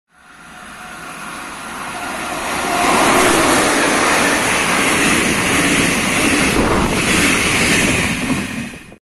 A 166-os Szili teper a TULIPÁN IC-vel Szolnok felé
Igen, ez egy igazán hosszúra sikerült videó... :)
Mozdony: 431 166
Kocsik: Bd, Bpee, Bpee, Bpee, Bbdpmz, Apee,
Vonatszám: 568
A vonat 15:30 Budapest-Keleti pu. - Budapest-Nyugati pu. 21:42 viszonylatban közlekedett.
A videó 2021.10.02-án 19:49-kor készült.